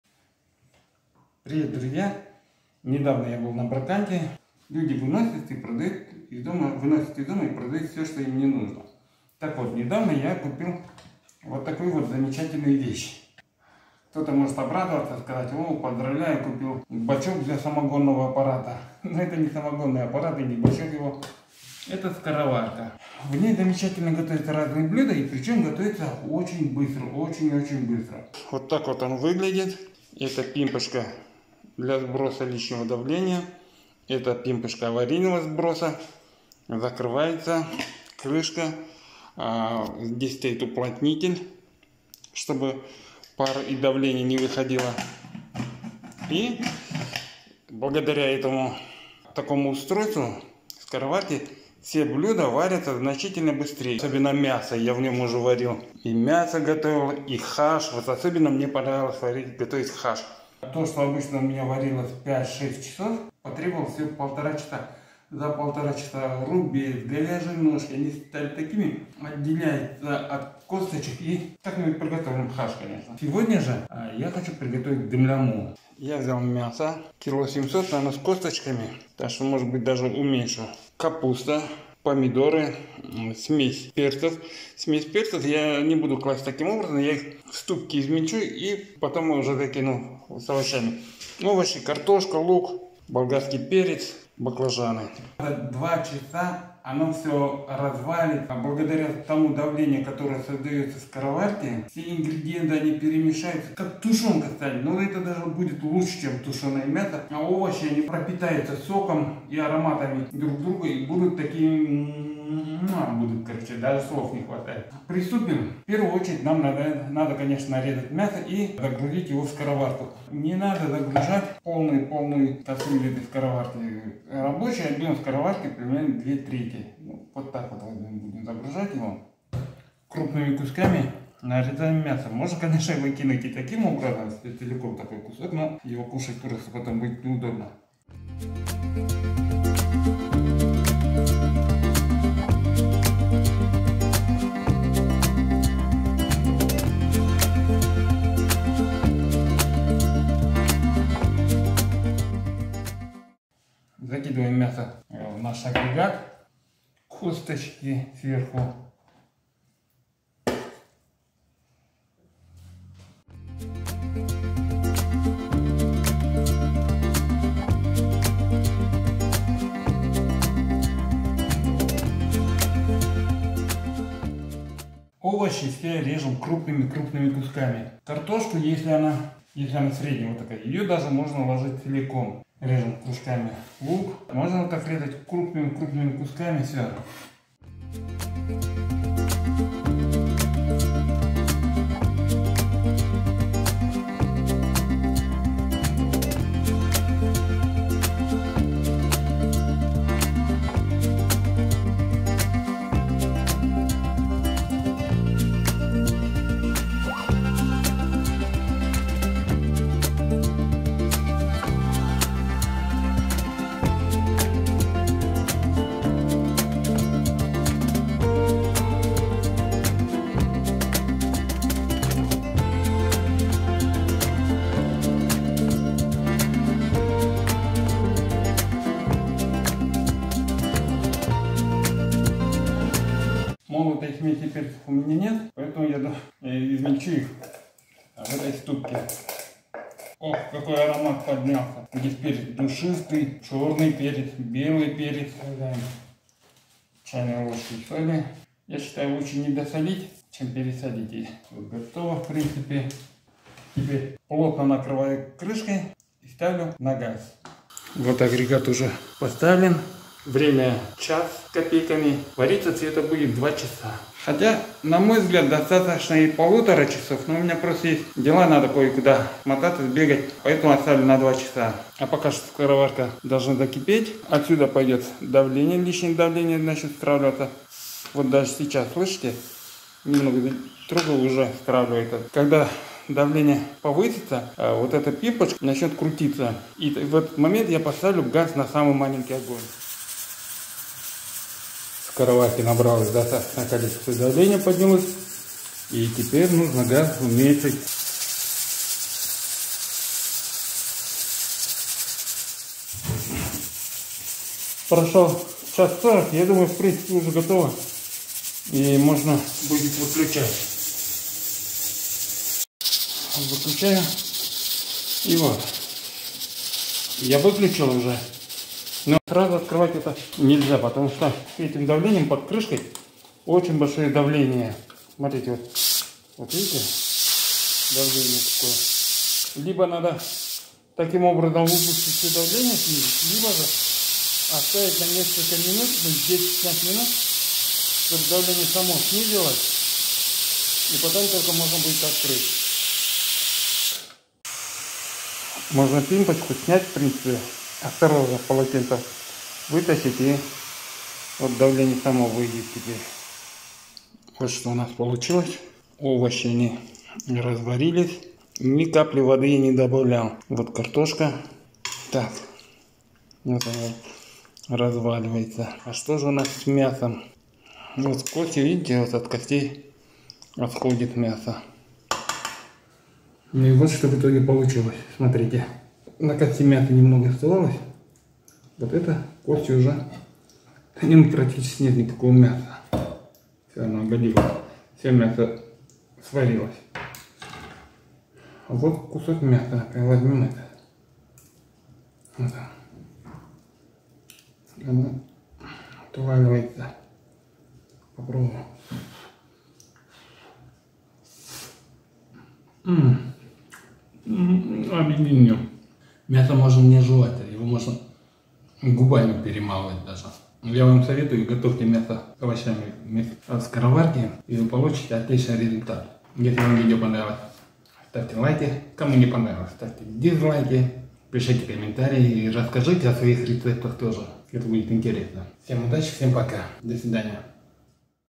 МЯСО С ОВОЩАМИ В СКОРОВАРКЕ. ПРОСТОЙ И ВКУСНЫЙ РЕЦЕПТ. Приготовьте это блюдо, другого вы не захотите
Купил на одном из брокантов (блошиных рынков) скороварку всего за 8 евро. В магазине такой агрегат стоит не меньше 80 евро. Но не зависимо от цены аппарата, не поверите, блюда заиграли новыми вкусами. Хотел приготовить дымляму, но не все овощи не поместились, в итоге получилось просто тушеные овощи с мясом. Вкус просто обалденный.

Предлагайте свои рецепты блюд в скороварке :) :)